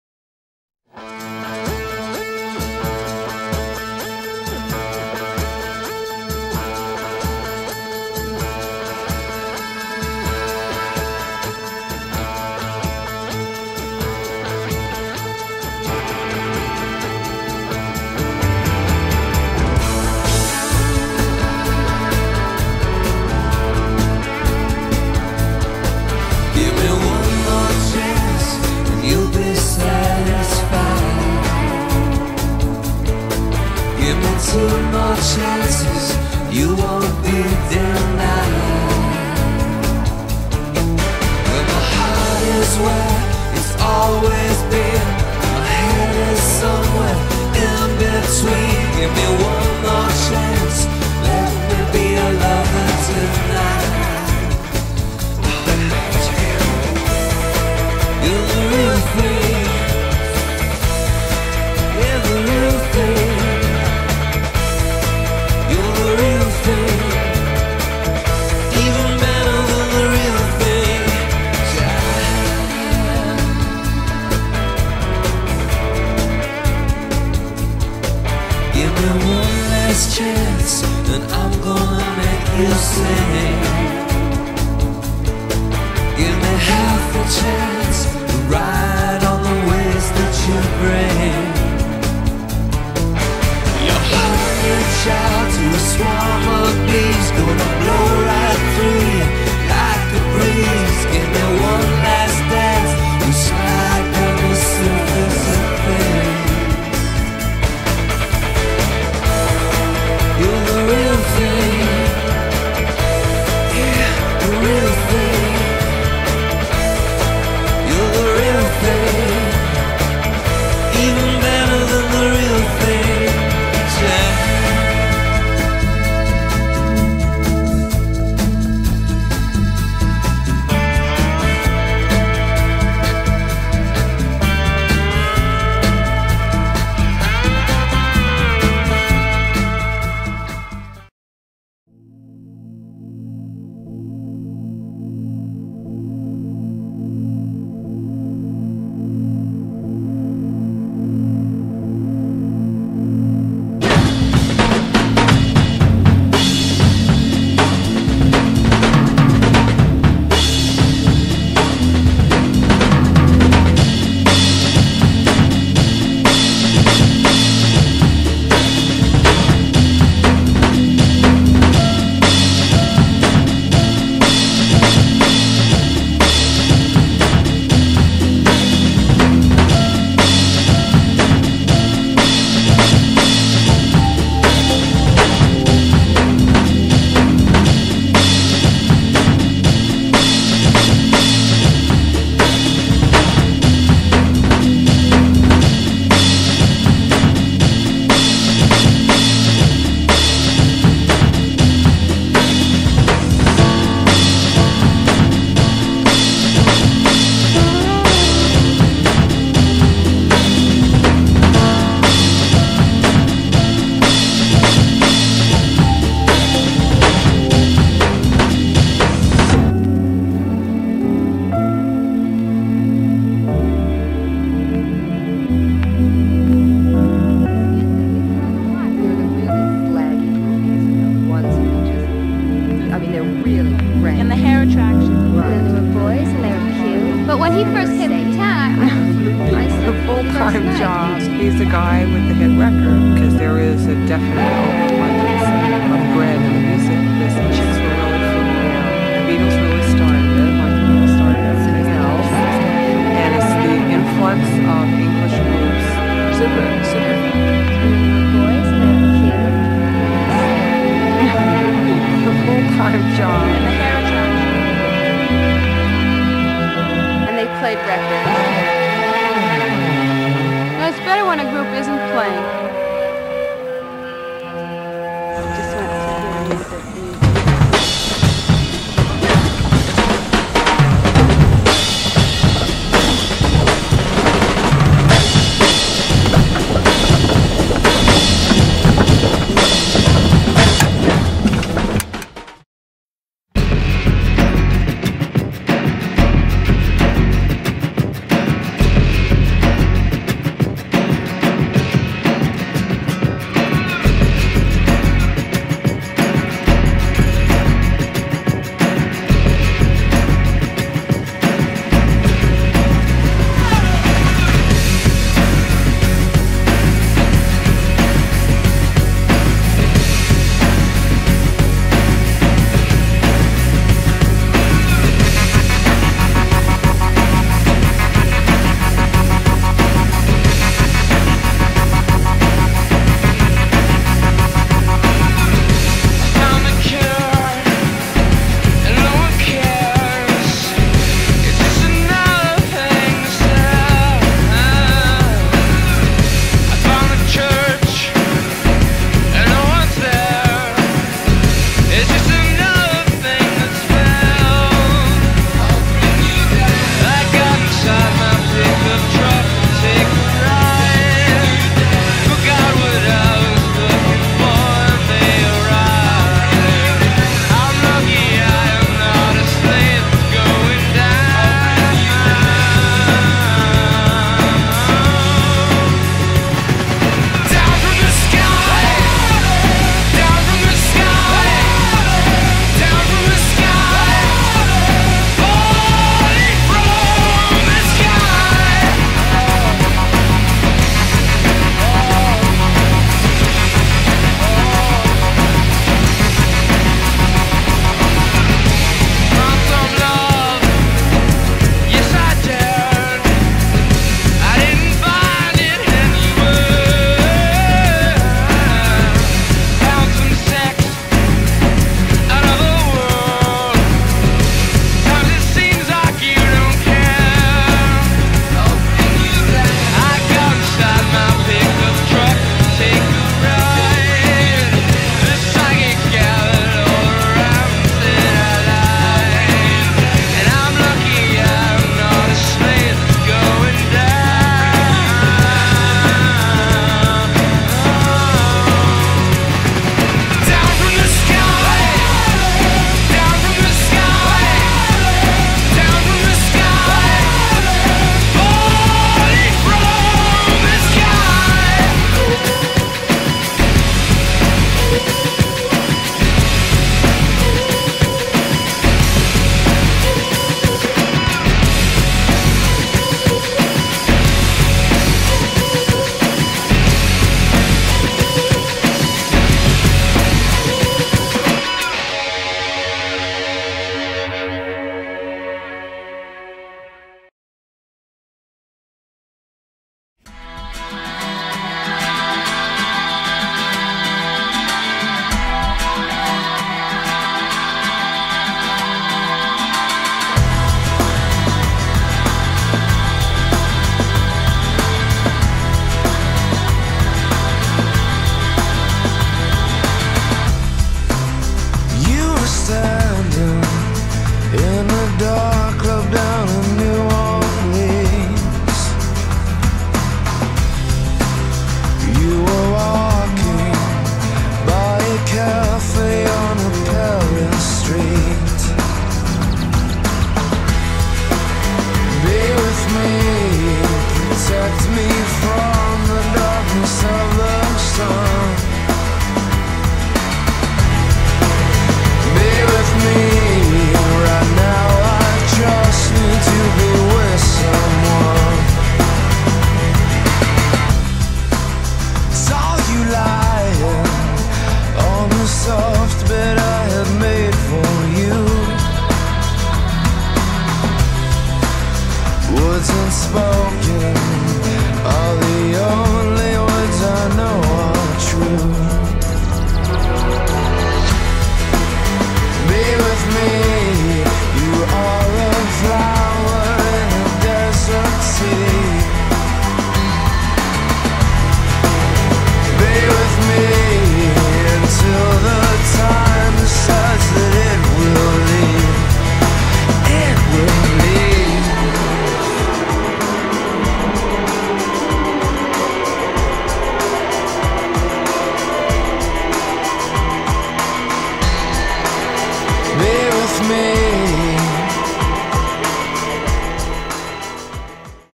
The Real Deal
Jeep JK Wrangler adventures in Maine 2008